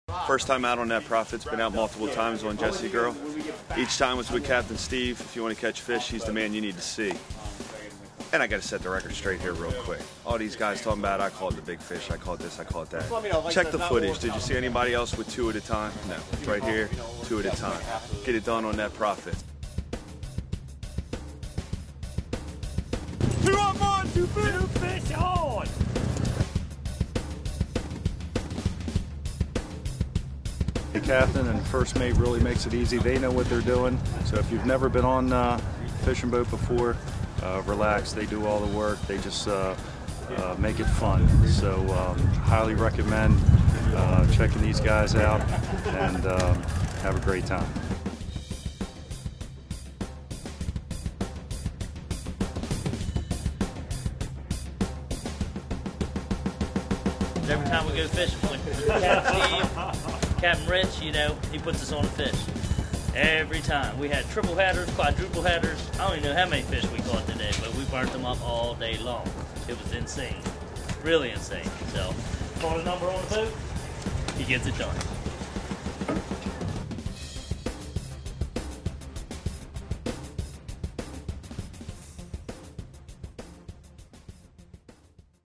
Testimonials from the Net Profits
Hear what clients fishing on the Chesapeake Bay aboard the Net Profits with Chesapeake Bay Sport Fishing have to say about their experience. More videos found @